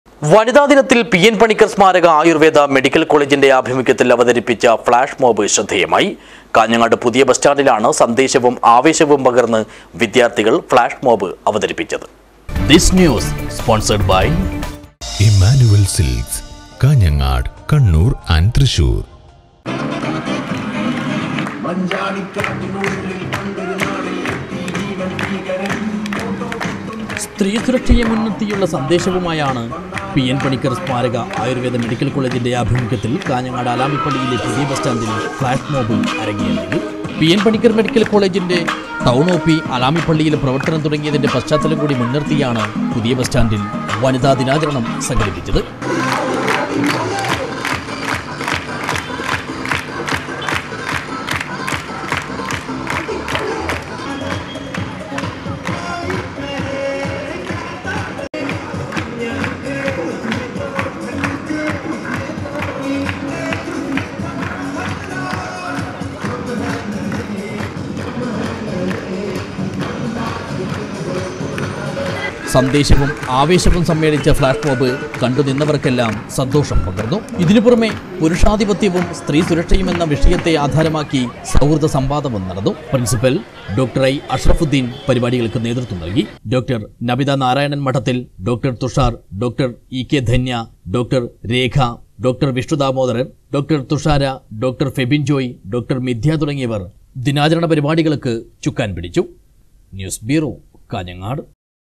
വനിതാ ദിനത്തില്‍ പി.എന്‍ പണിക്കര്‍ സ്മാരക ആയൂര്‍വ്വേദ മെഡിക്കല്‍ കോളേജിന്റെ ഫ്‌ളാഷ് മോബ്
വനിതാ ദിനത്തില്‍ പി.എന്‍ പണിക്കര്‍ സ്മാരക ആയൂര്‍വ്വേദ മെഡിക്കല്‍ കോളേജിന്റെ ആഭിമുഖ്യത്തില്‍ അവതരിപ്പിച്ച ഫ്‌ളാഷ് മോബ് ശ്രദ്ധേയമായി. കാഞ്ഞങ്ങാട് പുതിയ ബസ്റ്റാന്റിലാണ് സന്ദേശവും ആവേശവും പകര്‍ന്ന് വിദ്യാര്‍ത്ഥികള്‍ ഫ്‌ളാഷ് മോബ് അവതരിപ്പിച്ചത്.